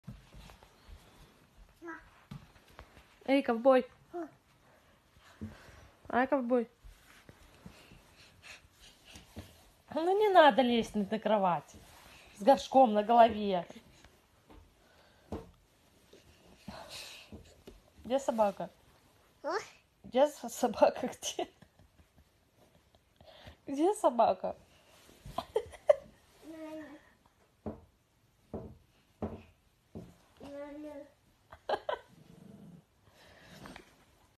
Hilarious toddler wears toilet seat on head like cowboy hat
A video of a toddler wearing a toilet seat on his head like a cowboy hat has gone viral on social media. The video was taken on April 15, 2022 in Mga village, Russia and features a boy named Timofey.